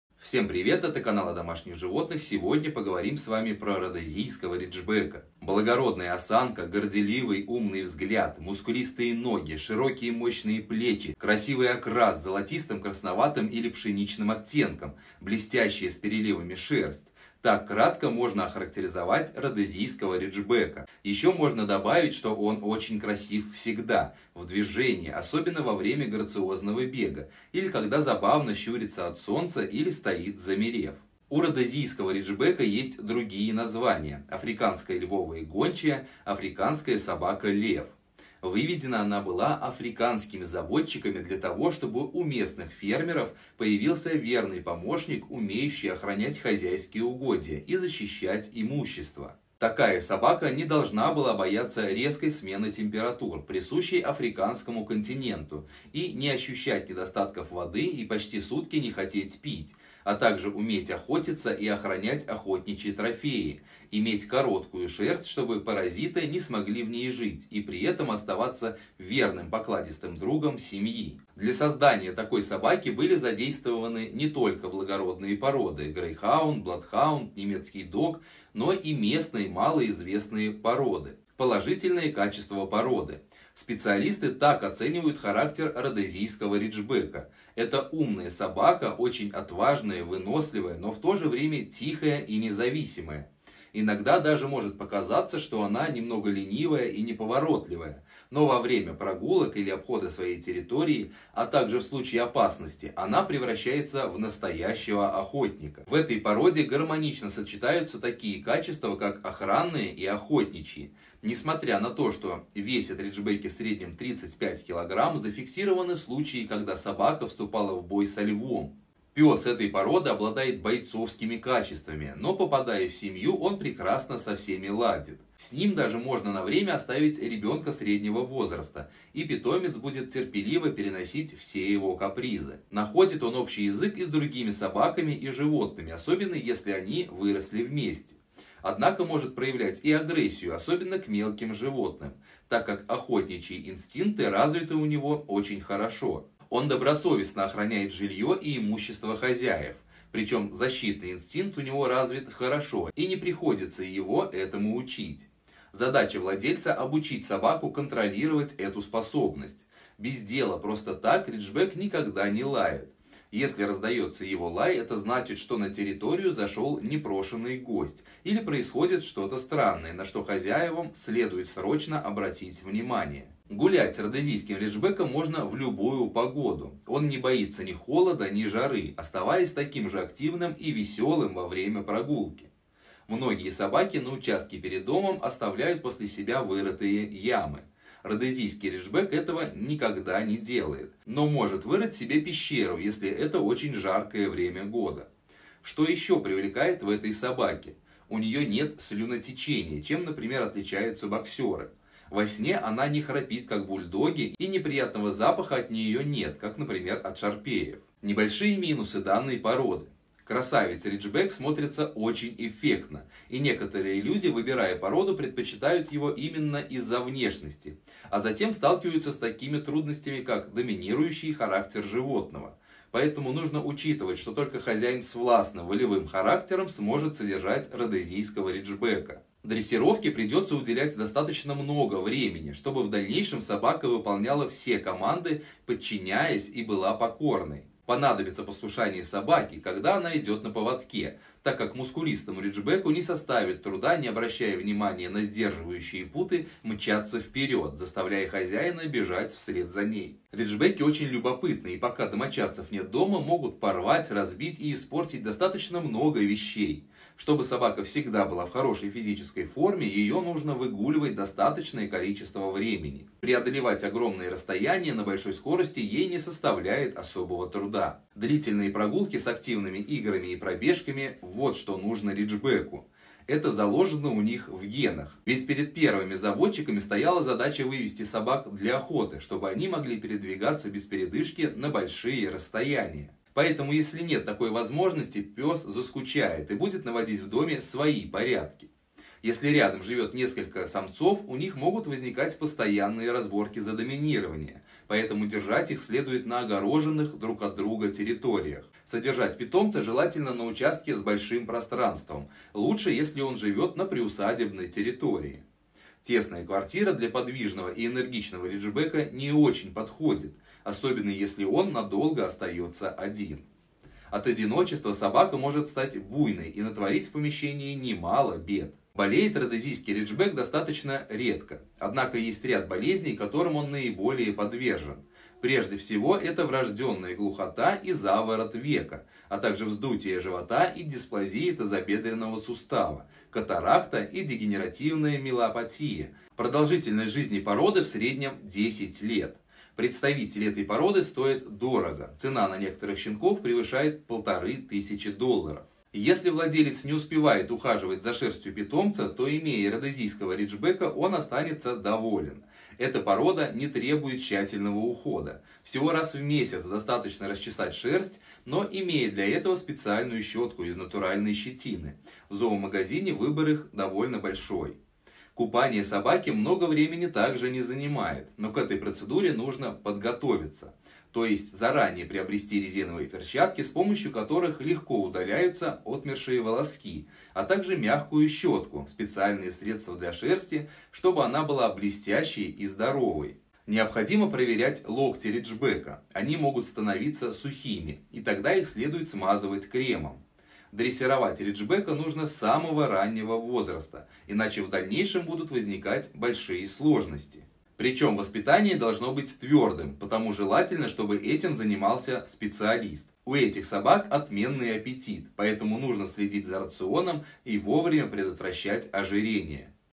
Rhodesian Ridgeback | Character, care and education 2022
The Rhodesian Ridgeback is a large sighthound dog breed bred in the Southern Africa region.[1] Its forebears can be traced to the semi-domesticated ridged hunting and guardian dogs of the Khoikhoi. These were interbred with European dogs by the early colonists of the Cape Colony of southern Africa. The original breed standard was drafted by F. R. Barnes, in Bulawayo, Southern Rhodesia (now Zimbabwe), in 1922, and approved by the South African Kennel Union in 1927. The Rhodesian Ridgeback at present is the only registered breed indigenous to Southern Africa.